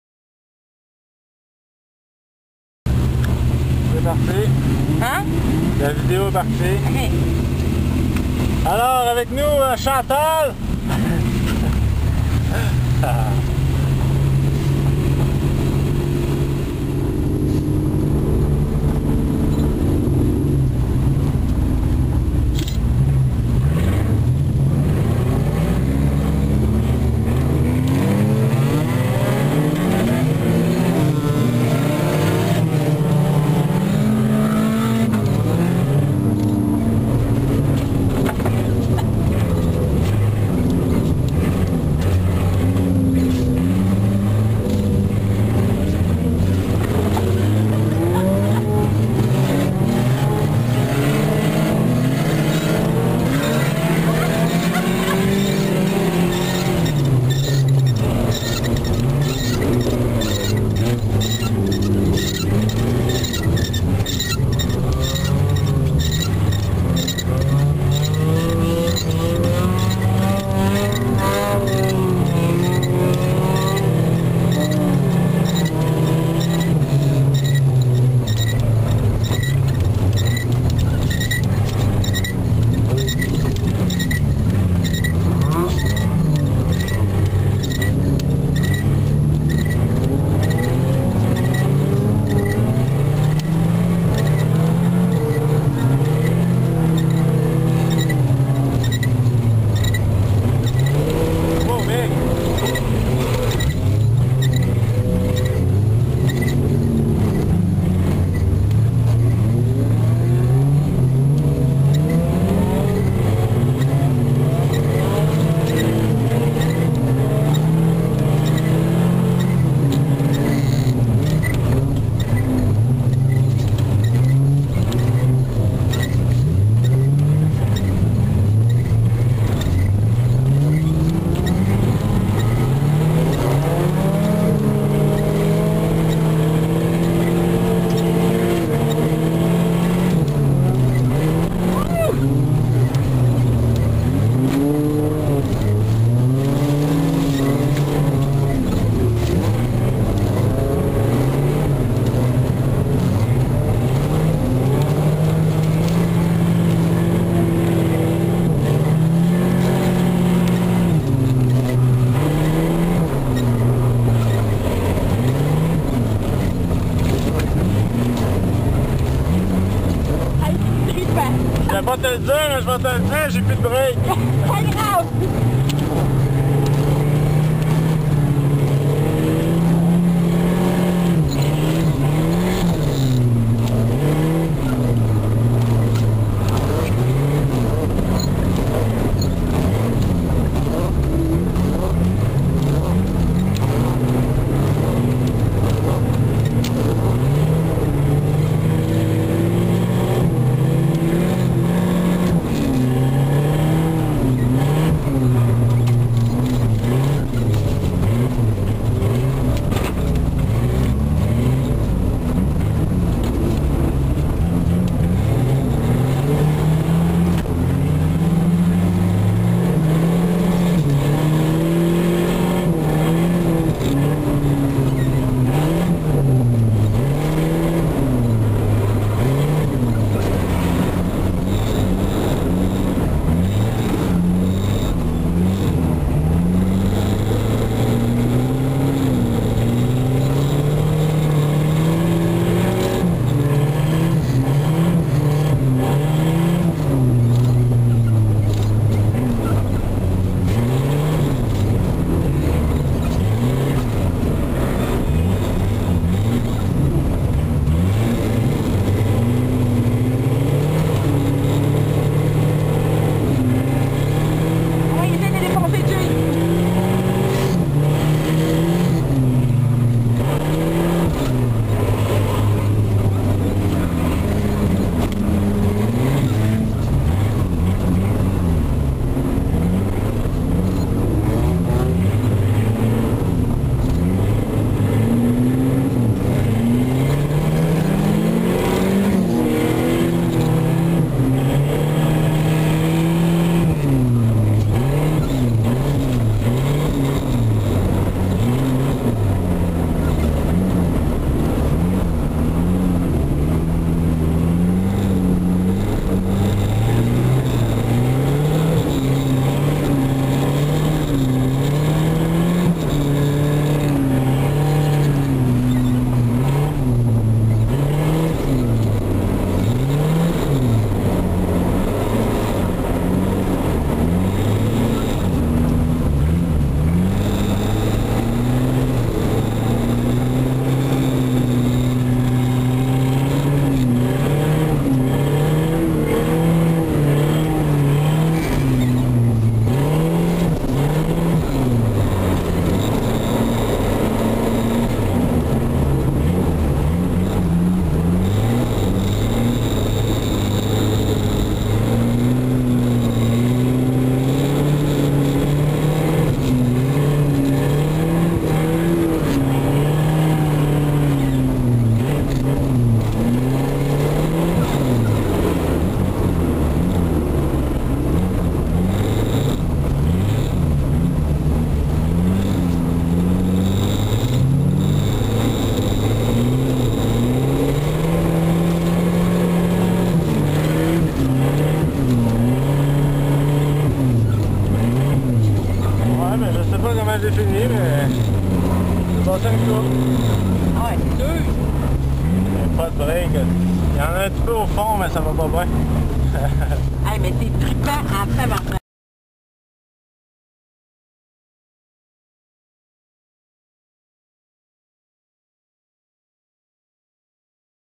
qualif INSA 16-01-11
la qualif avec Chantale Boisvert comme 1ere expérience en copilote :)